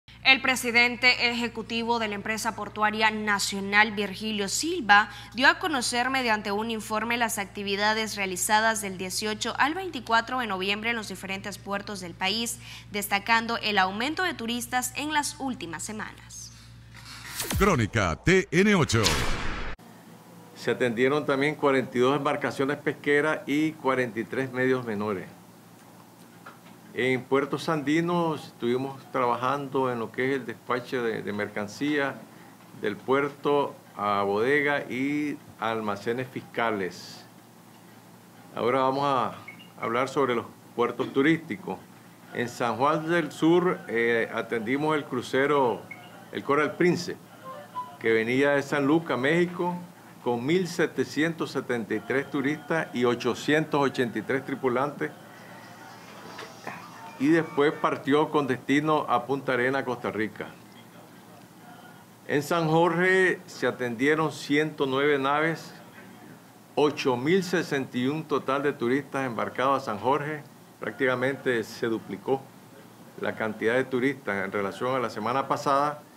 Puertos continúan fortaleciendo la economía nicaragüense
El presidente ejecutivo de la Empresa Portuaria Nacional (EPN), Virgilio Silva, dio a conocer mediante un informe las actividades realizadas del 18 al 24 de noviembre en los diferentes puertos de Nicaragua.